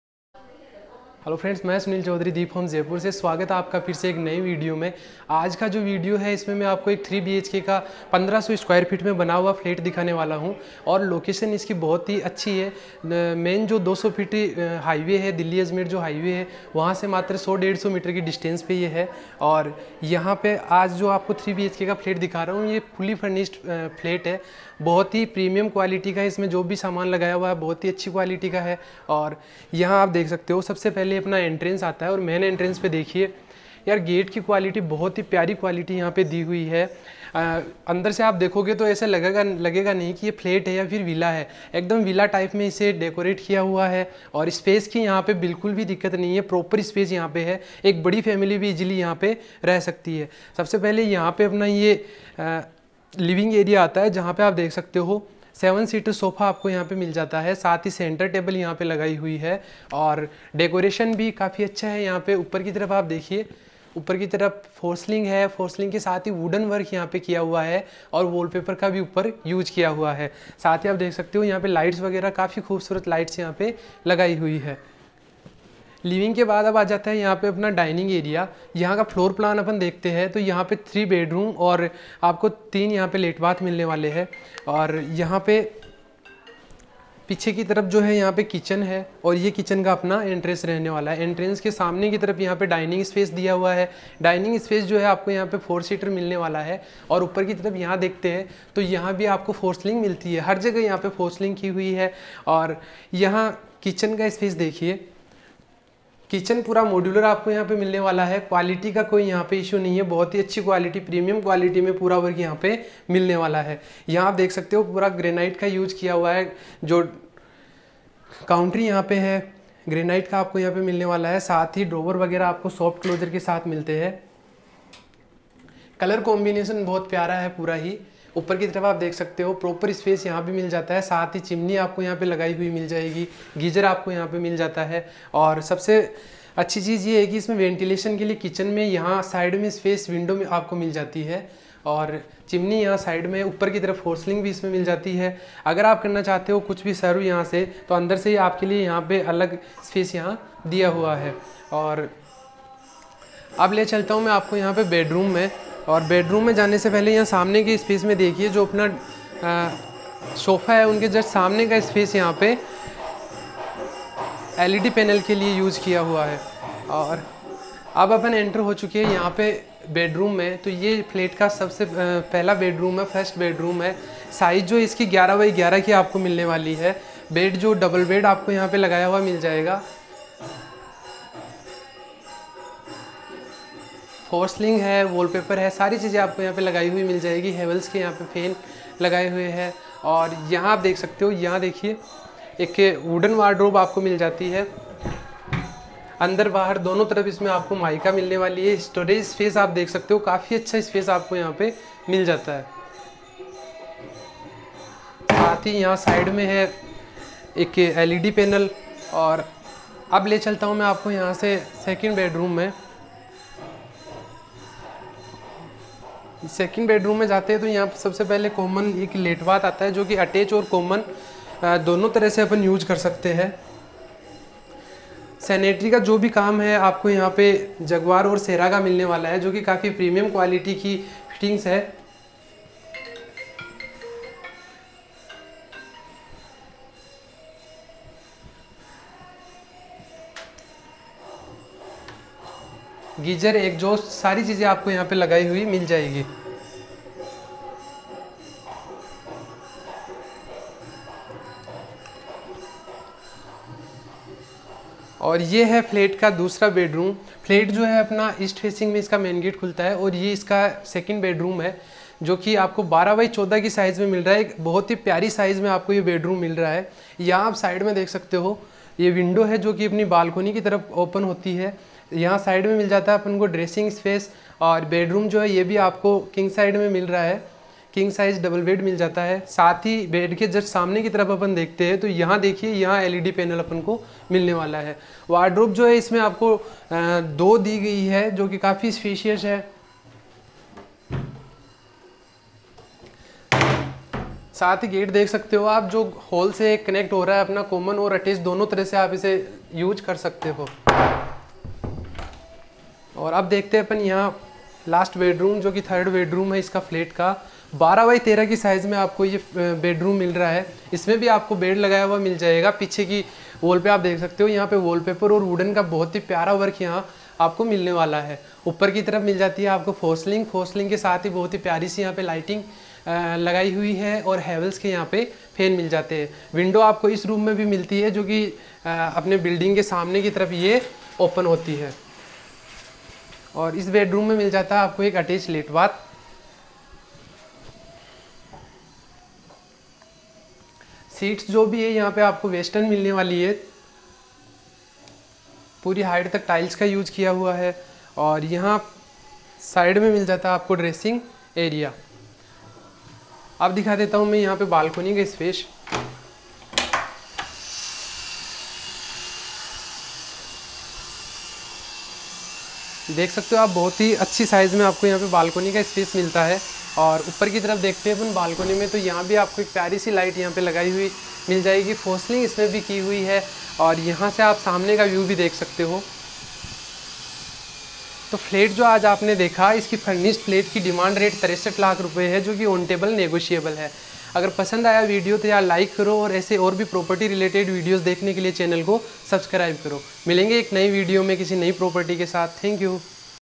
वैशाली नगर की प्राइम लोकेशन गांधीपथ पर 1500 sq ft मे बना lavish flat | 3 Bhk fully furnished flat
3Bhk fully furnished luxury flat for sale in vaishali nagar jaipur
3 bhk luxury flat in jaipur
3bhk 1500sqft flat for sale in jaipur
Prime location of gandhi path vaishali nagar jaipur
asking price  63 lac

Jda approved property
Modular kitchen Wooden wardrobes tubellite fans AC points
Furinshed flat
Area 1500 sqft
Location - gandhi path west, vaishali nagar jaipur rajasthan